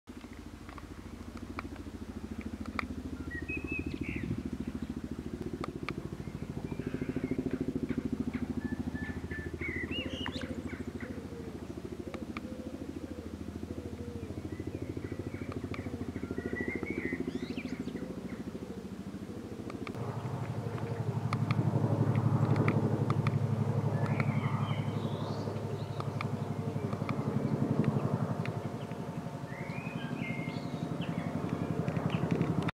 RAF Chinnock flying over North Norfolk 21/06/22
A lone RAF Chinnock flying over North Norfolk and heads eventually for Norwich Airport for refuelling.